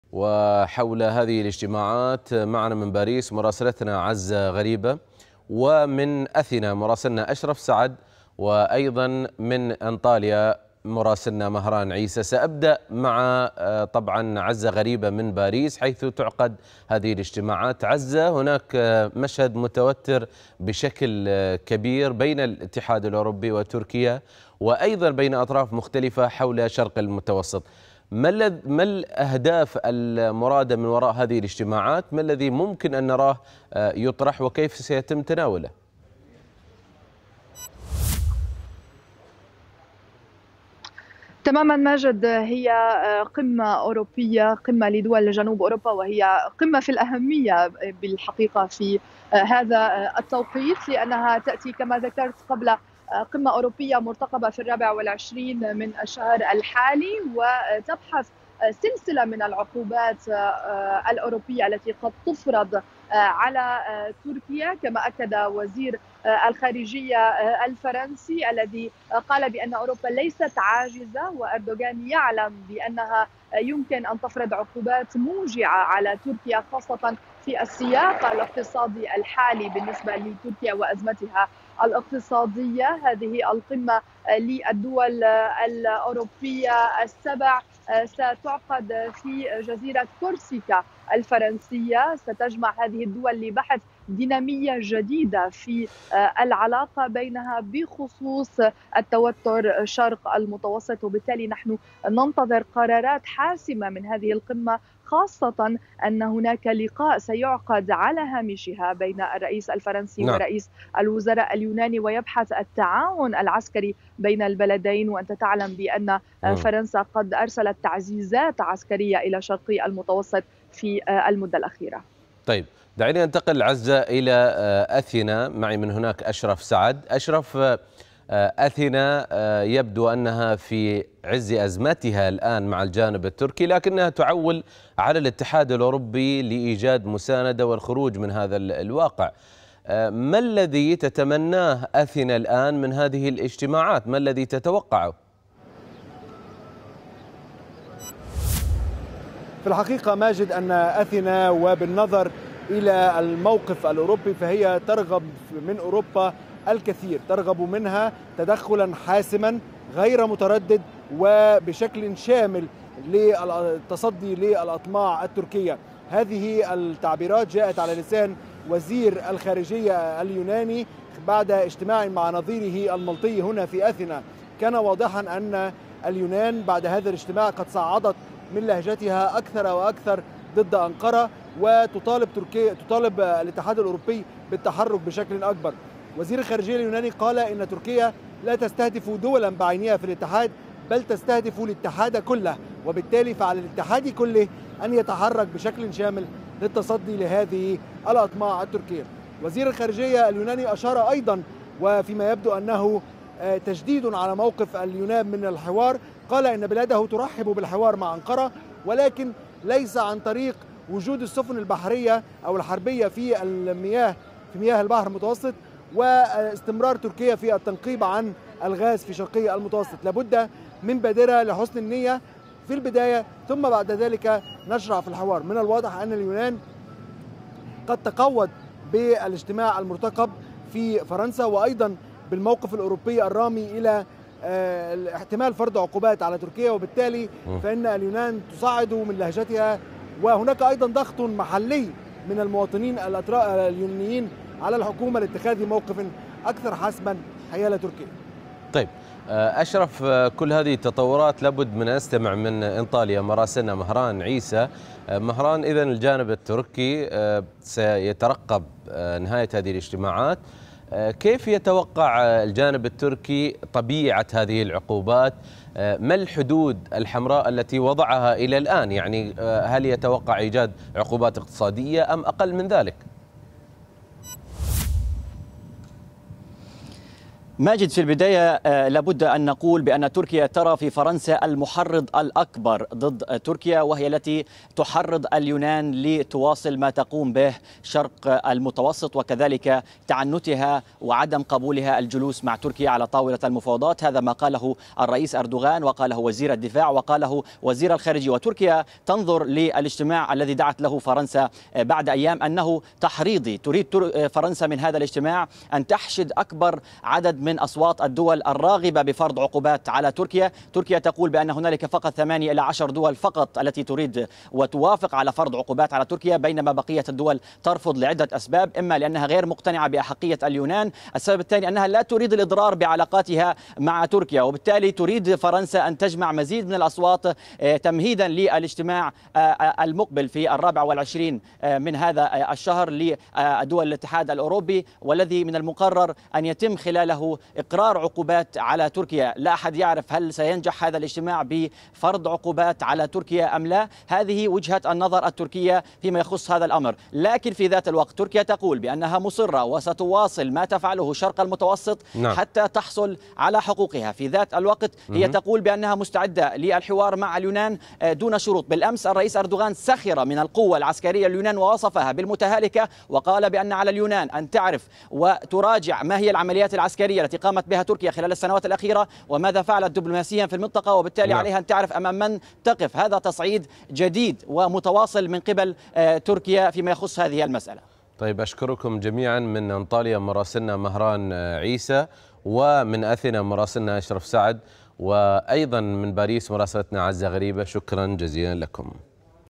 فرنسا تستضيف قمة سباعية لبحث التوتر في شرق المتوسط.. شاهد التفاصيل
تستضيف فرنسا اجتماعا لمجموعة دول المتوسط الأوروبية السبع، الخميس المقبل، لبحث التوتر مع تركيا، بينما اتهم وزير خارجية اليونان تركيا بأنها تنتهك ميثاق الأمم المتحدة. ويتصاعد التوتر شرقي البحر المتوسط، حيث هناك اندفاع تركي، واحتشاد عسكري يوناني، واستنفار سياسي أوروبي، بينما تغيب التكهنات التي تنبئ بقرب الوصول إلى حلول وسط، تنزع فتيل صراع الغاز، في أخطر بقعة مائية.. مزيد من التفاصيل يتحدث مراسلو سكاي نيوز عربية، من باريس عزة غريبة، من أثينا أشرف سعد، ومن أنطاليا مهران عيسى.